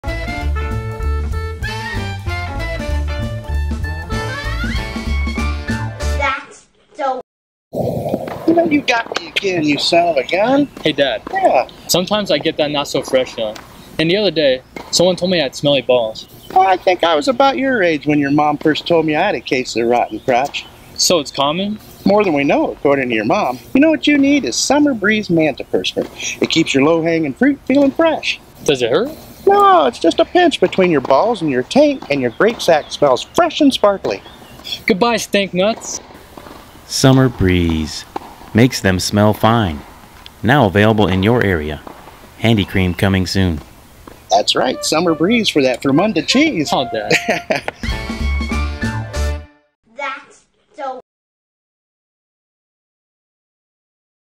Summer Breeze - for when men get that not so fresh feeling
Calling all men... Ever get that not so fresh feeling down there? Get yourself some Summer Breeze. The woman in your life will thank you.